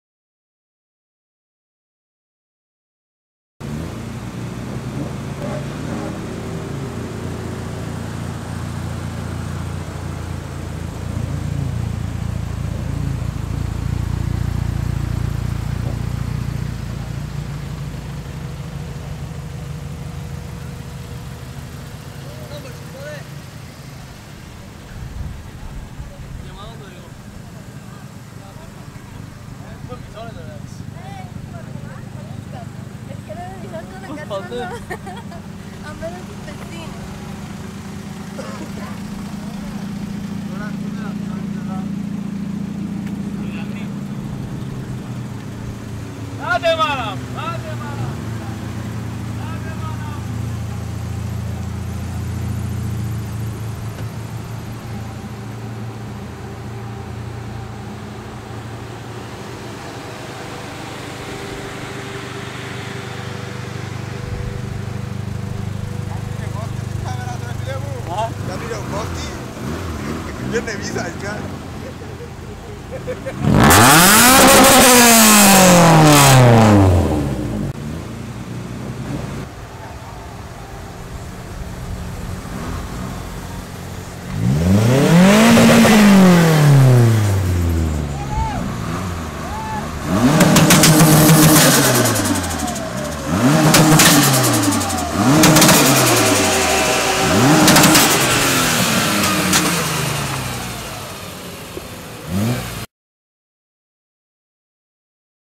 Cyprus Sport Car Club Event on 29.04.2012
Cyprus Sport Car Club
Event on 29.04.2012
Proetimasia gia tin poria pros Lemeso